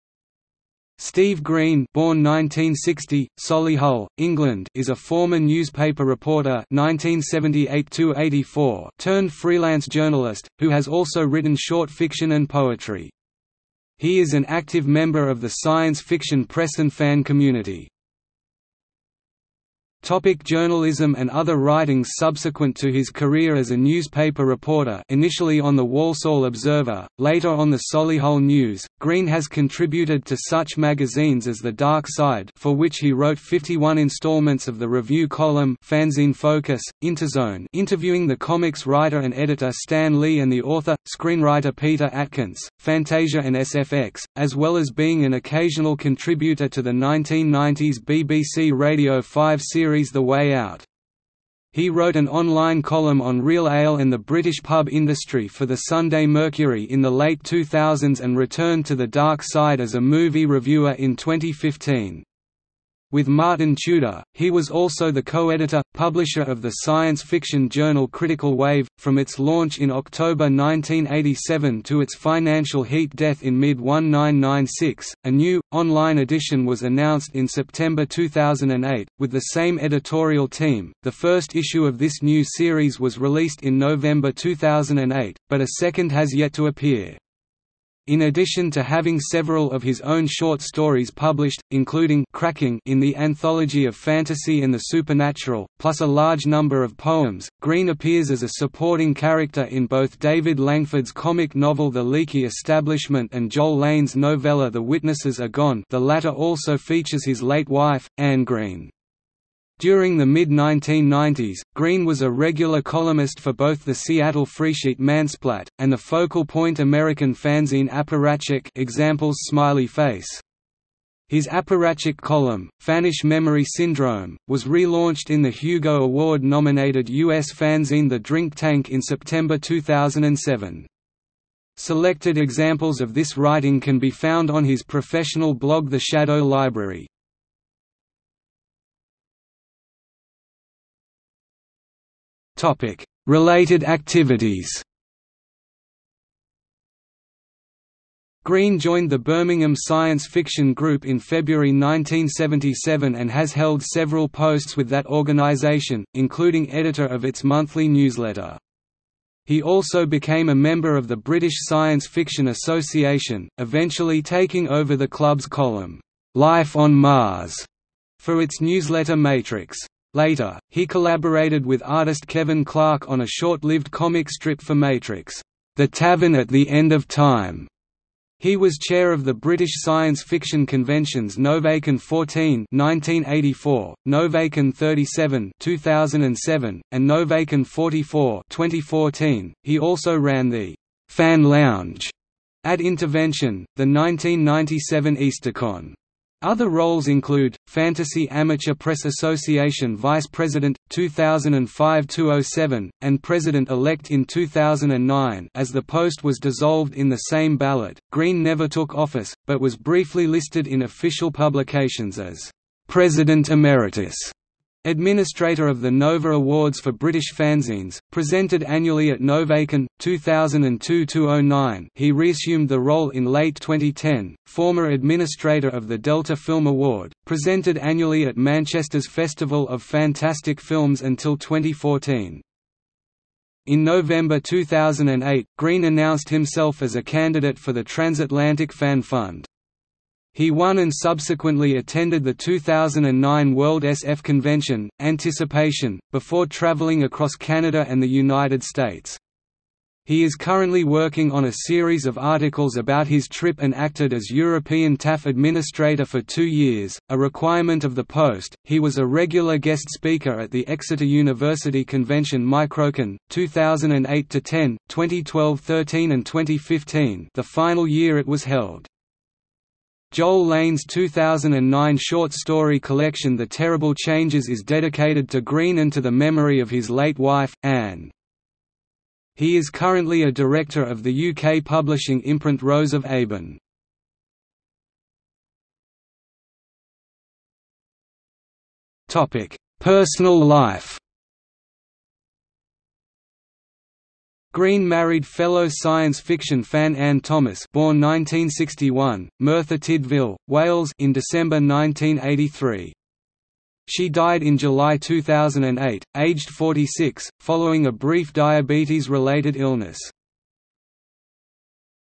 Steve Green (journalist) | Wikipedia audio article
00:00:19 1 Journalism and other writings
00:02:19 2 Related activities
00:04:45 3 Personal life

- Socrates

SUMMARY
Steve Green (born 1960, Solihull, England) is a former newspaper reporter (1978–84) turned freelance journalist, who has also written short fiction and poetry. He is an active member of the science fiction press and fan community.